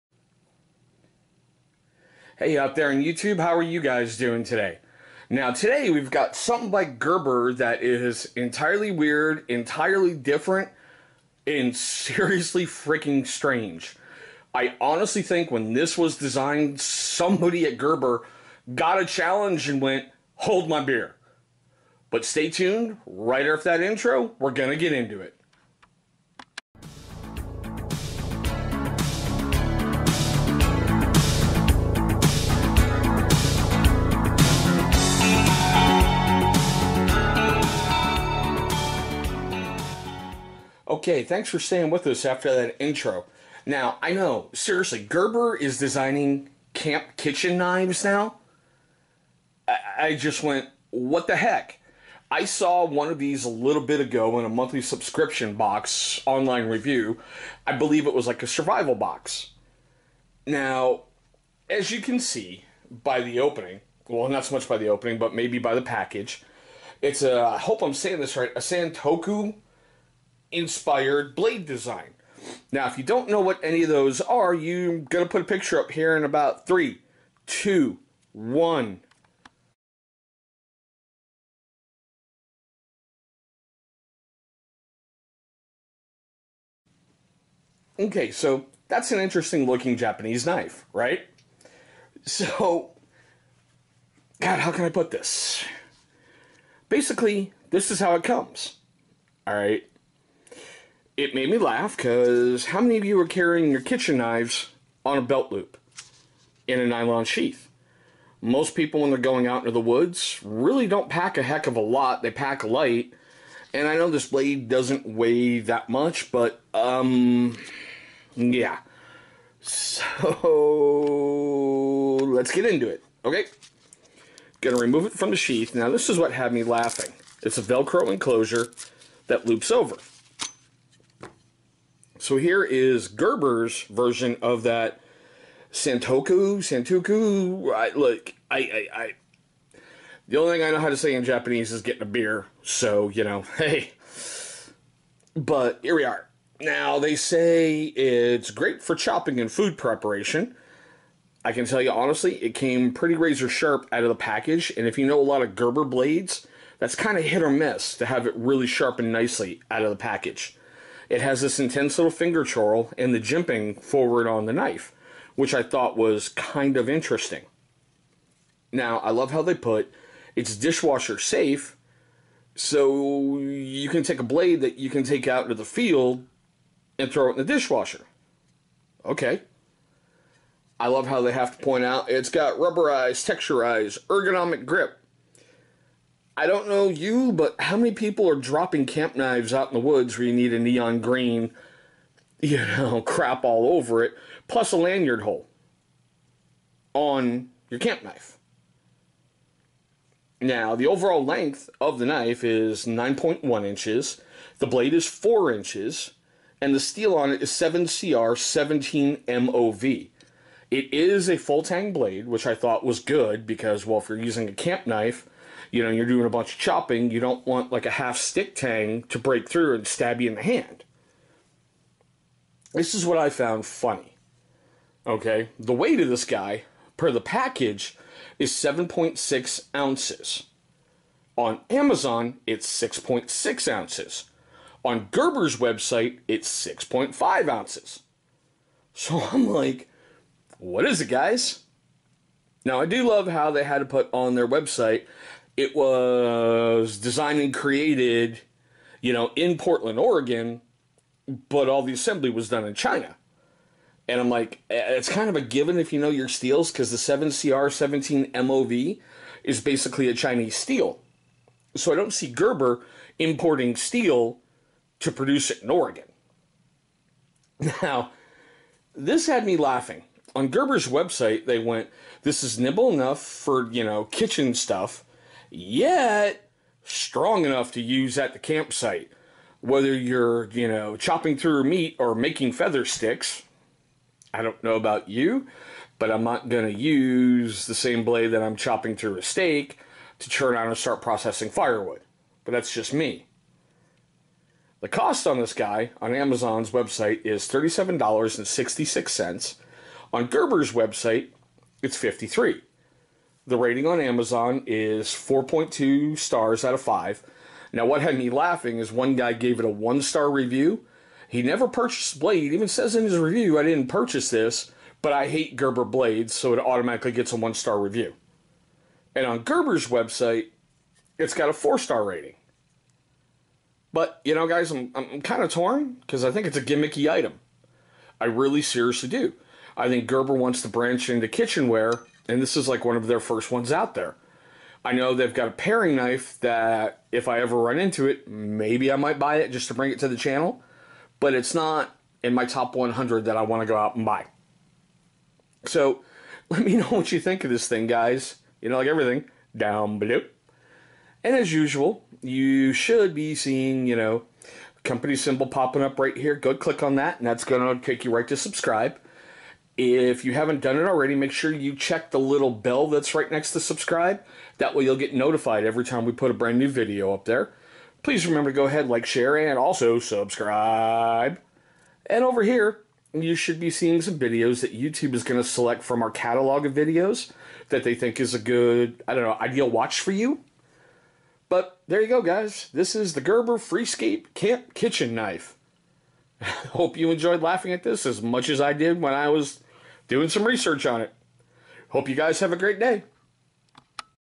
Gerber Freescape Camp Knife
Here is something kinda new from gerber. Its a combination of interesting, wrong and unusual all wrapped up in one. I know this will not be part of my EDC rotation, but i'm unsure if this will make its way into my BOB